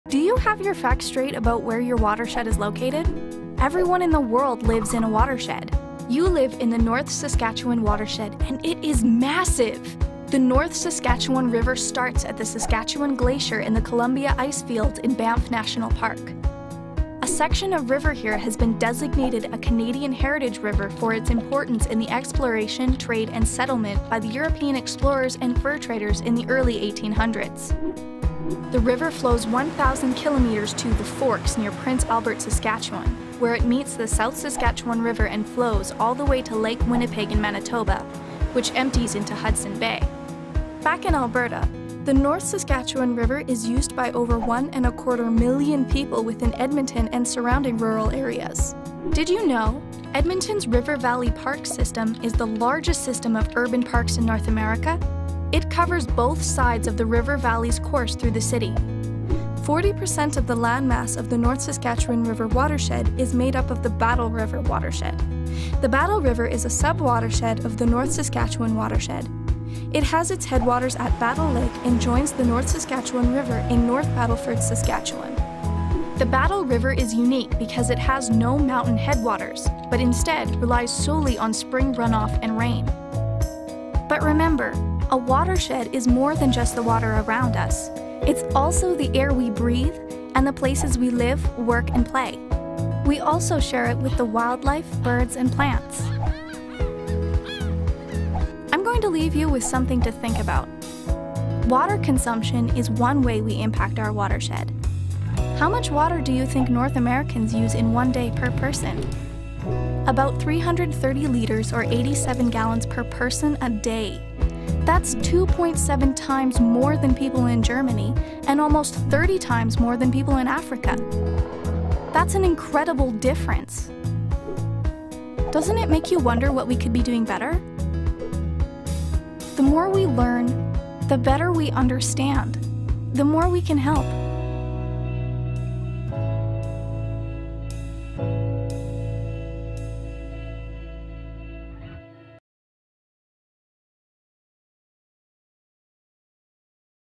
Where Is Your Watershed? (North Saskatchewan Watershed)
Do you have your facts straight about your watershed? This video covers lots of interesting facts about the North Saskatchewan, including the Battle River Watershed.